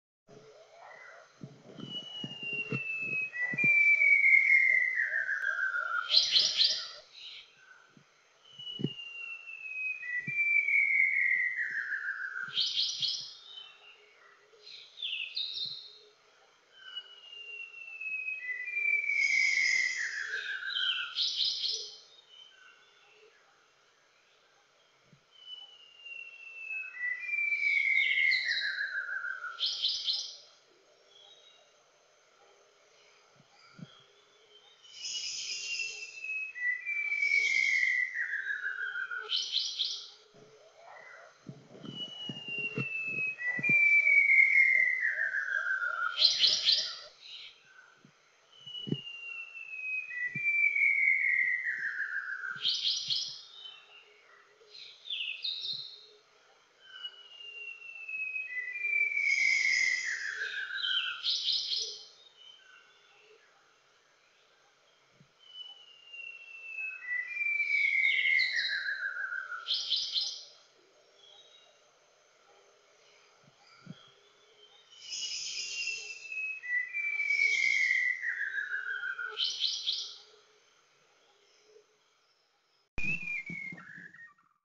White Browed Shama
An endemic species in Luzon, common but very shy, mostly heard but rarely seen. Adulting male filmed in Montalban, Rizal on Aug 2021